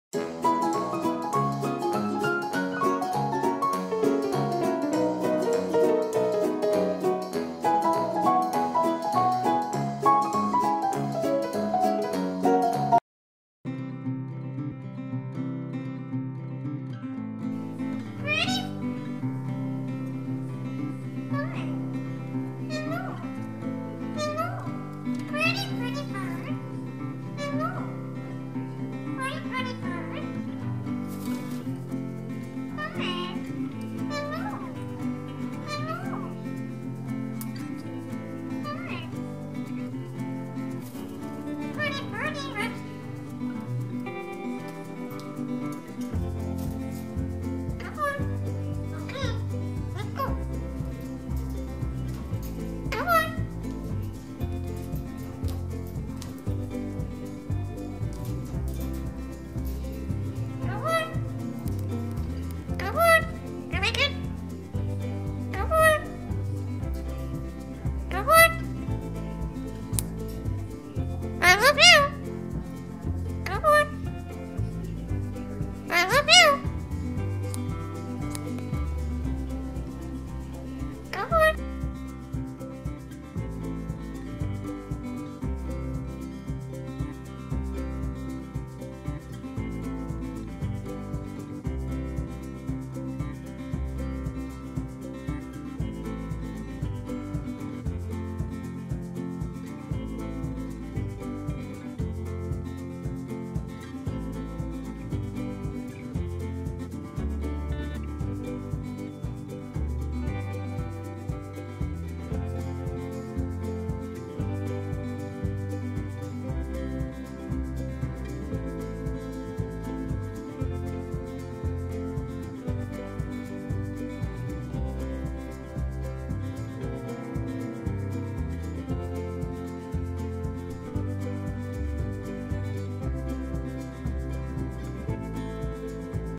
Indian Ring Neck Parakeet Live Nesting and Talking Mate
Indian Ring Neck Live Nesting and Talking Mate
We will keep you updated with the progress of the eggs and the growth of the baby birds when they arrive.

I hope you enjoy this video and check back as we take this journey with the baby eggs.